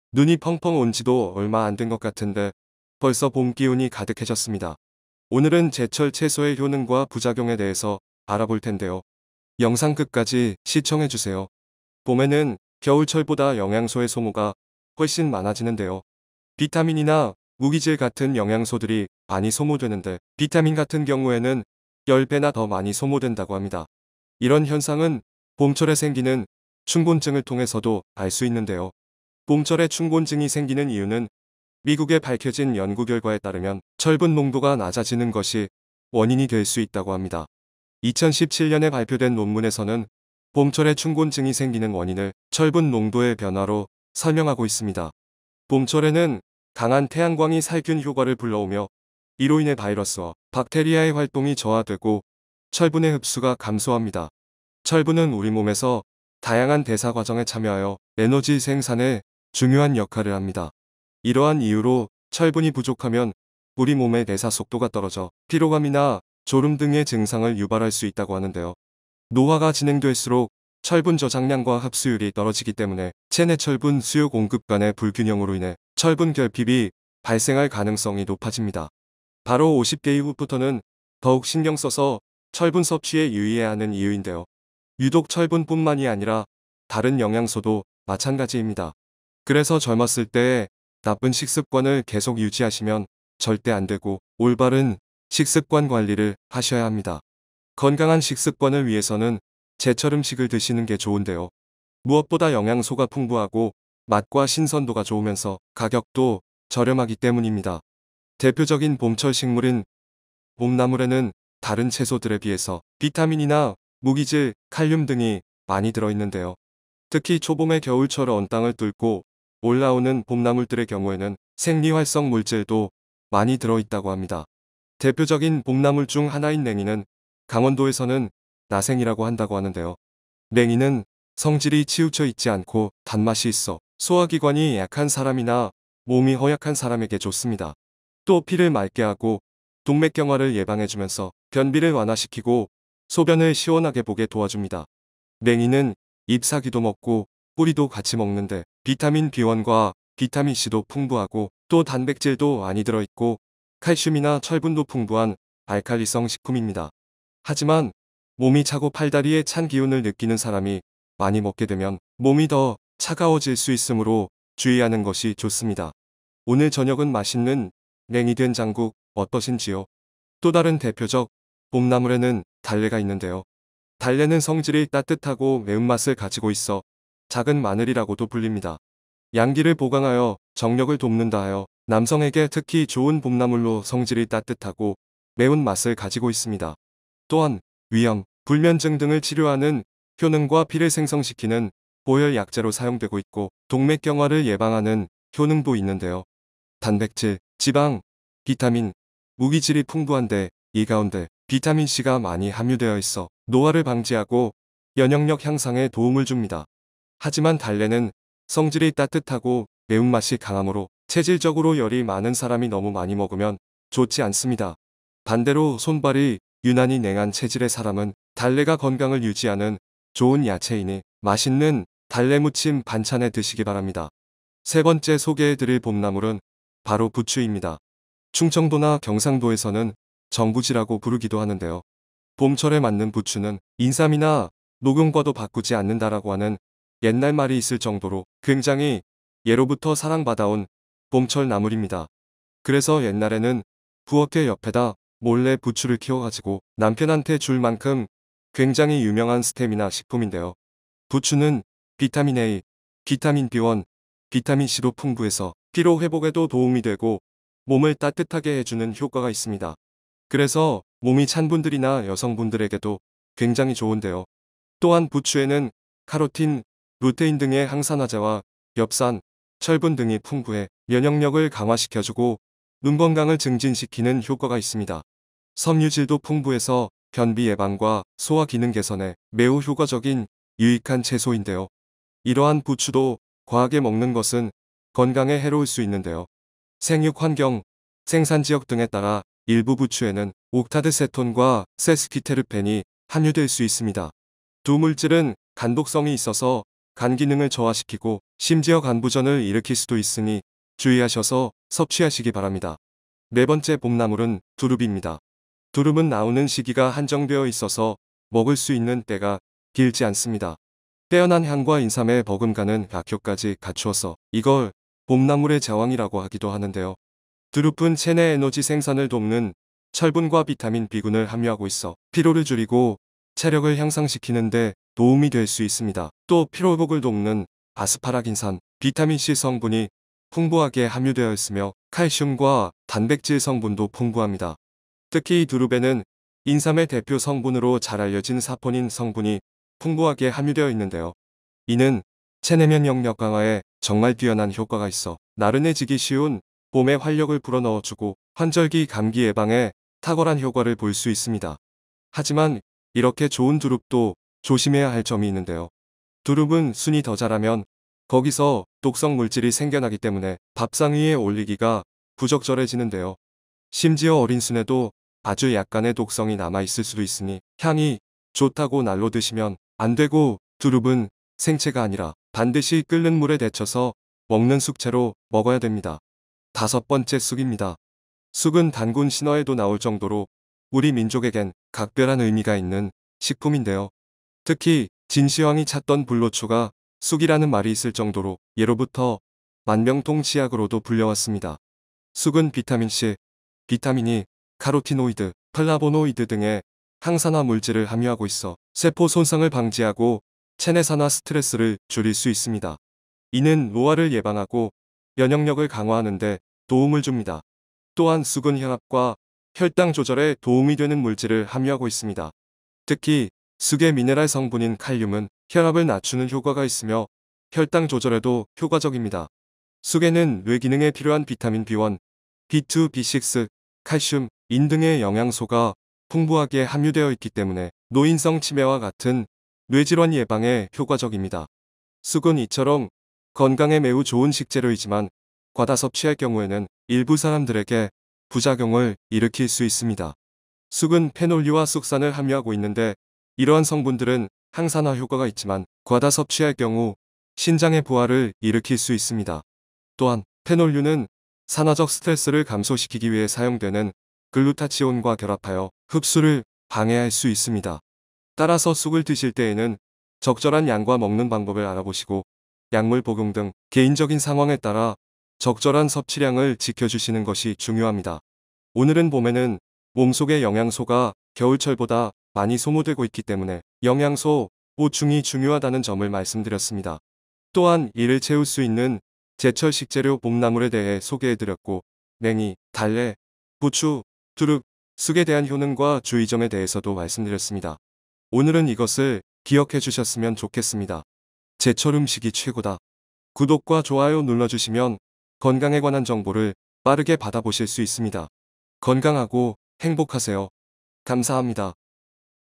50대 건강, 영양제 대신 먹을 수 있는 제철음식과 봄나물 추천! 비싼 보양식 먹기 전에, 봄나물과 제철음식을 드셔보세요 40대 50대 60대 70대 건강음식
50대 건강, 영양제 대신 먹을 수 있는 제철음식과 봄나물 추천
50대 건강을 위해 돈을 쓰지 마세요. 값싼 봄나물과 제철음식으로 충분합니다